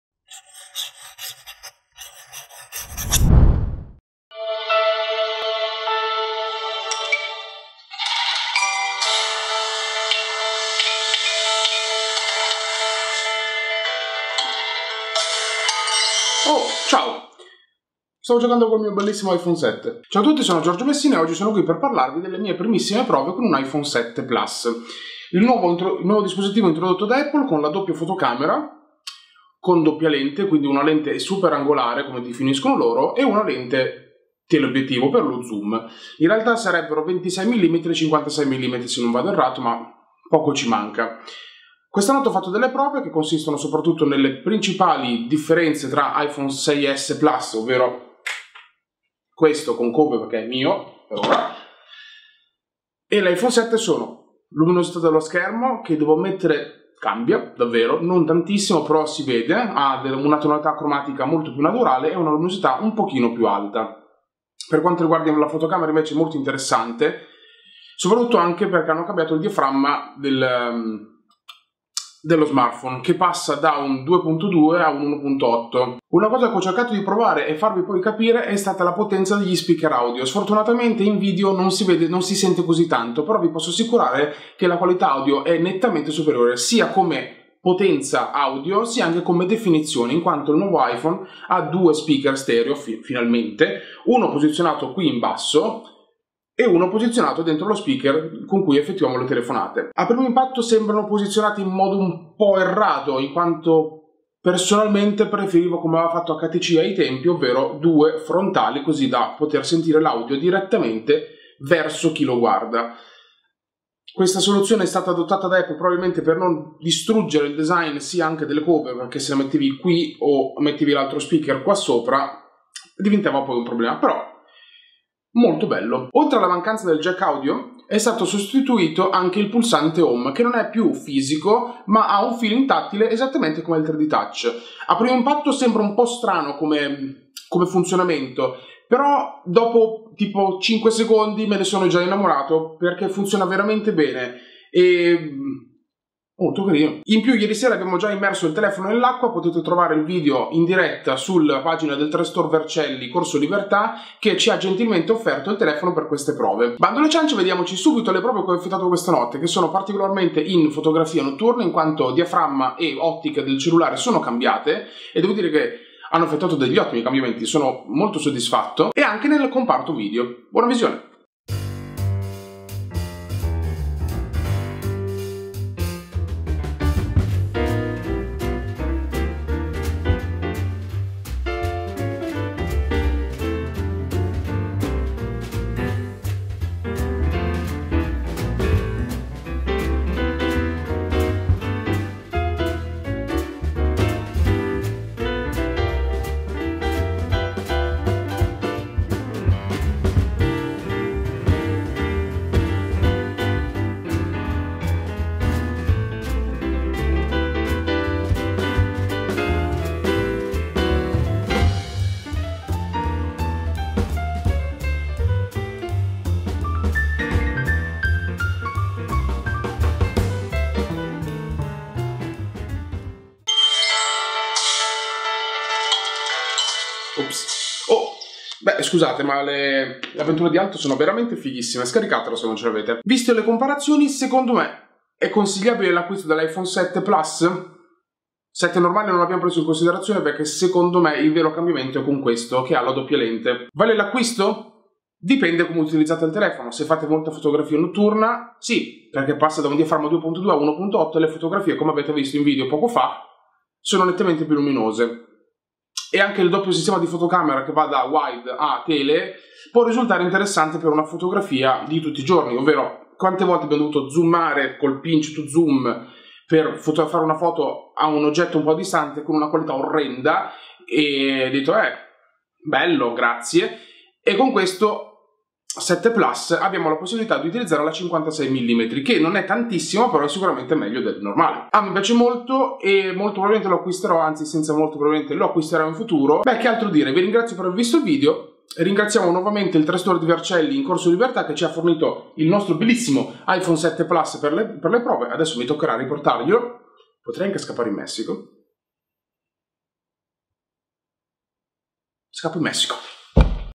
Prime impressioni iPhone 7 Plus
Prime prove ed impressioni con il nuovo iPhone 7 Plus.
Si ringrazia il 3Store corso Libertà 26 Vercelli per avermi fornito il dispositivo per i test.